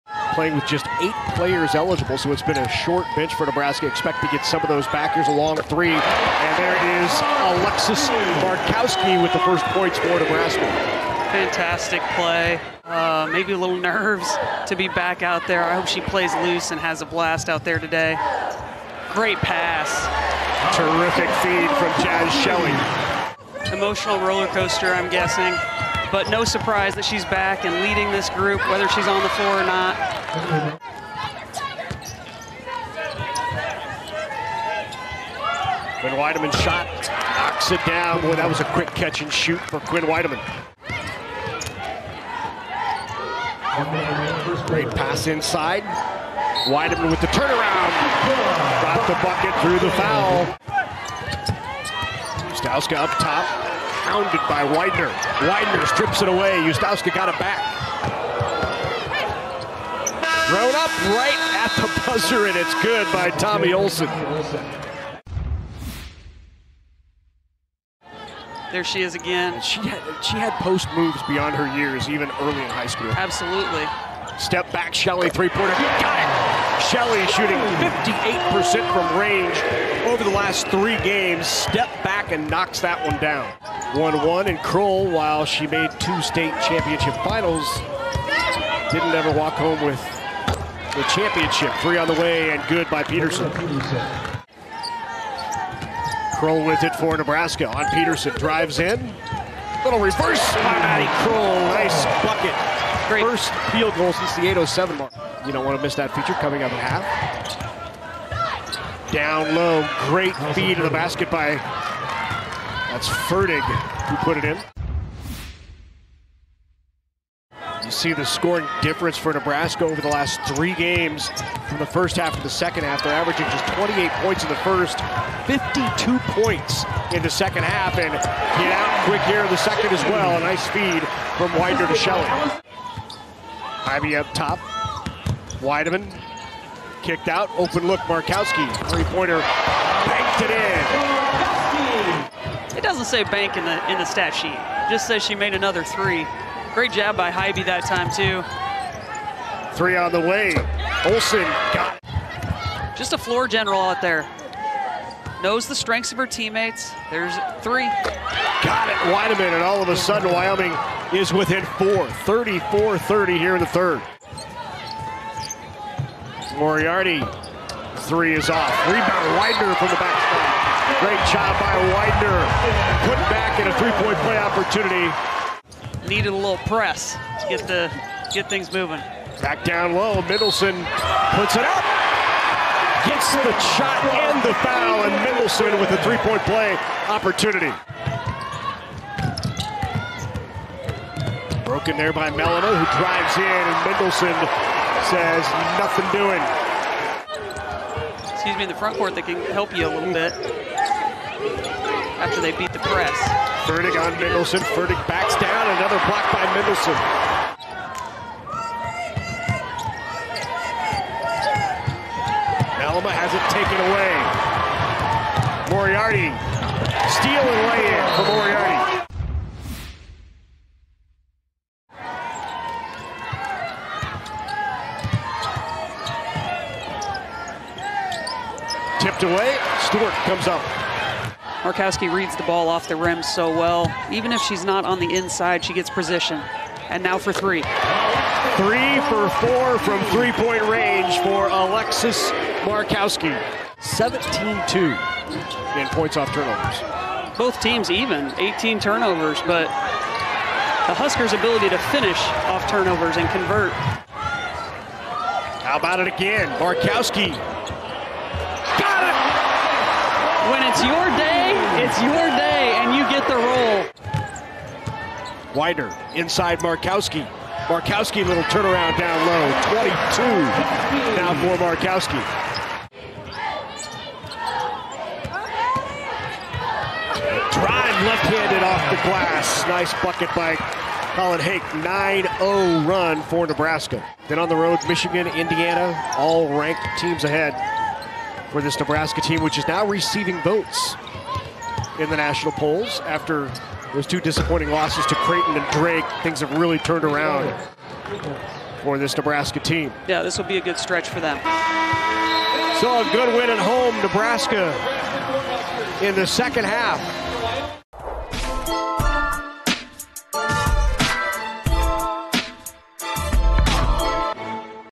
Wyoming at Nebraska | Highlights | Big Ten Women's Basketball | Dec. 18, 2022
00:00:00 - 1st Quarter
00:01:08 - 2nd Quarter
00:01:57 - 3rd Quarter
00:03:42 - 4th Quarter

00:00:00 - 1st Quarter
00:01:08 - 2nd Quarter
00:01:57 - 3rd Quarter
00:03:42 - 4th Quarter